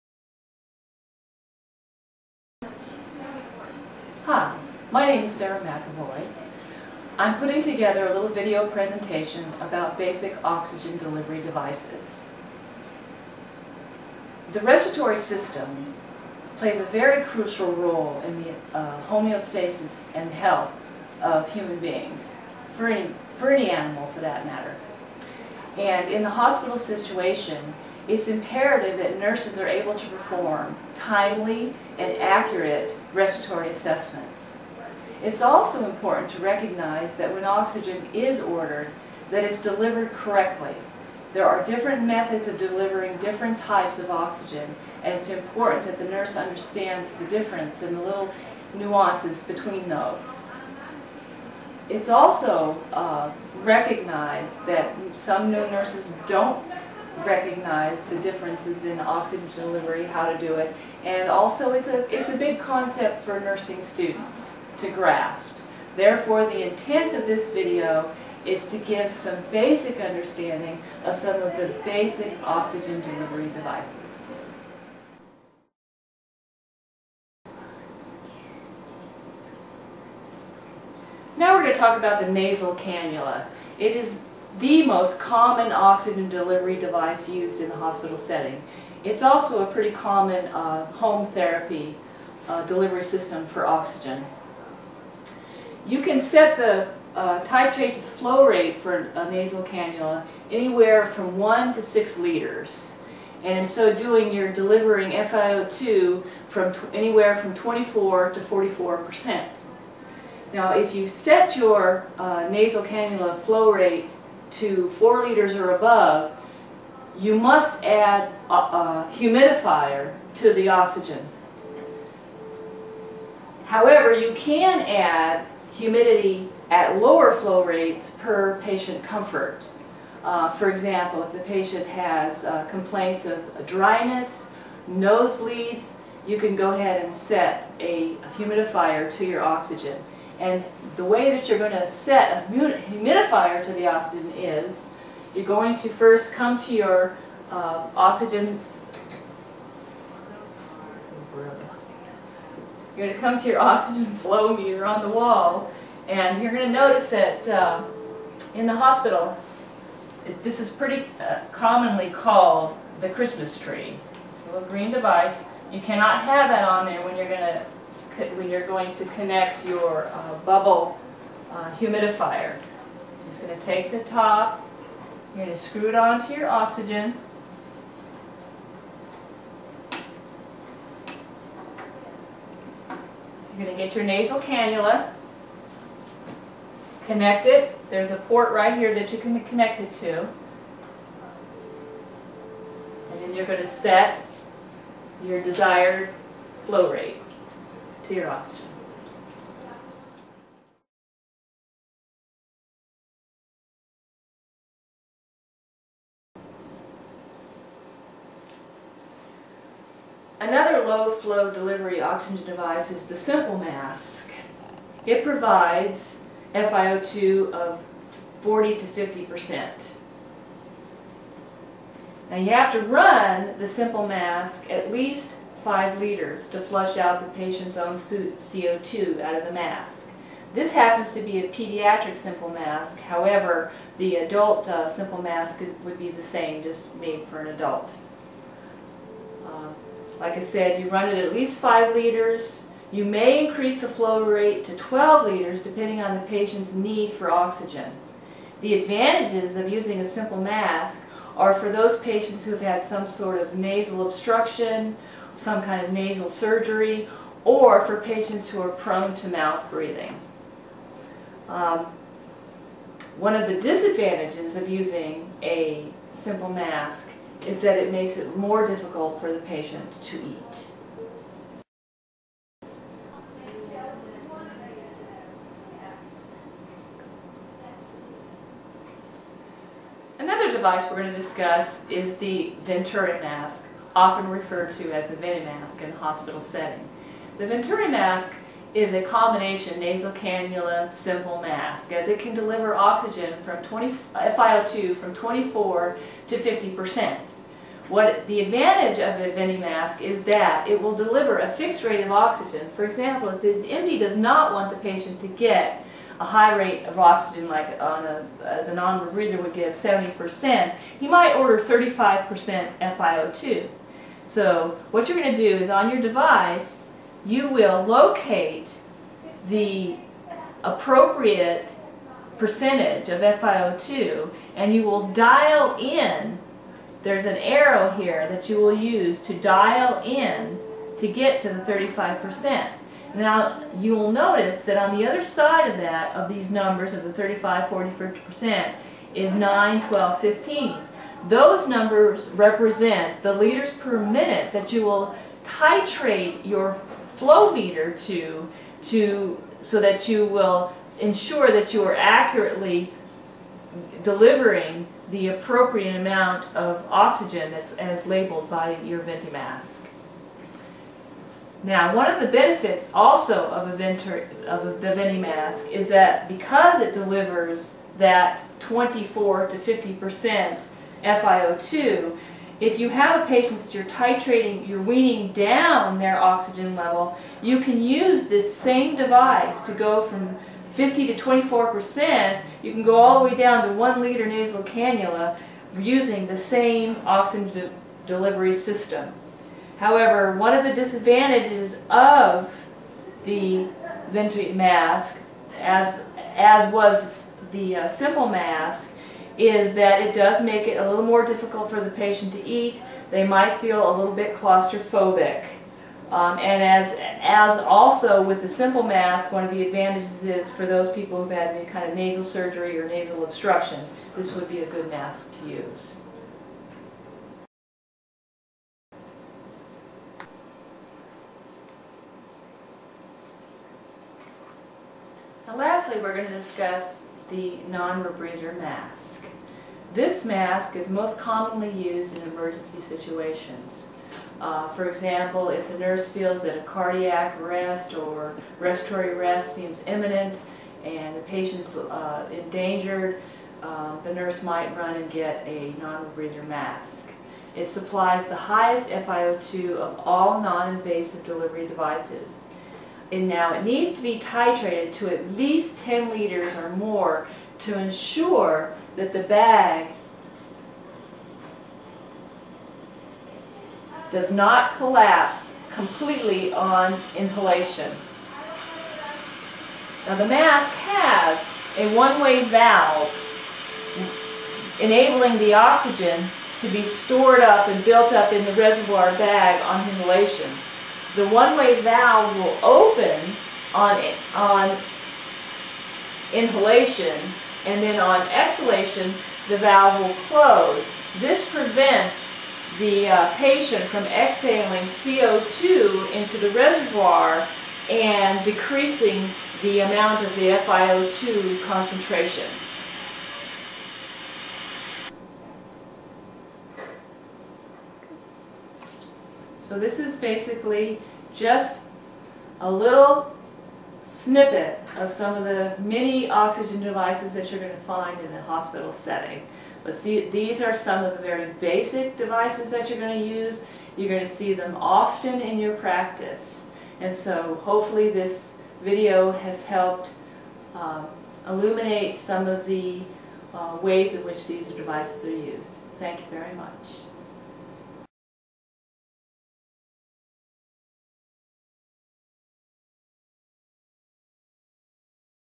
Basic Oxygen Delivery - NSG 651 - computer based teaching strategy
School project Podcast - teaching basic oxygen delivery devices